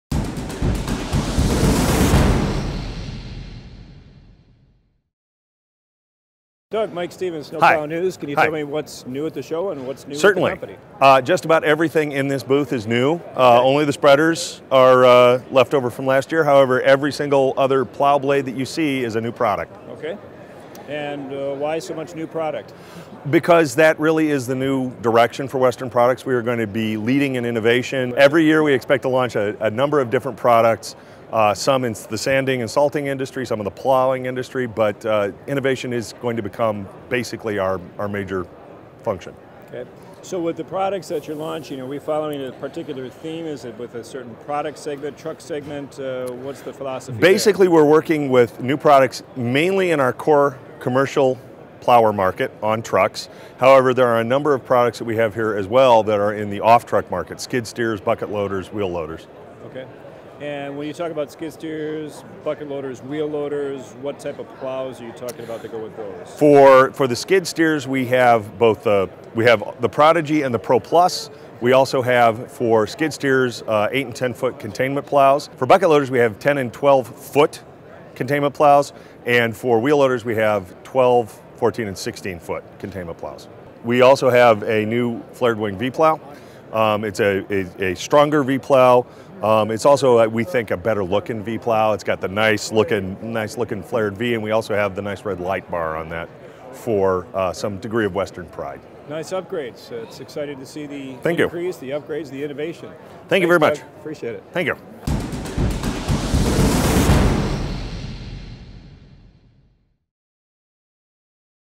Western Products sets Innovation Strategy
Western Products has made innovation a key strategy going into the future to supply its customers with what they need while they try to dominate the industry.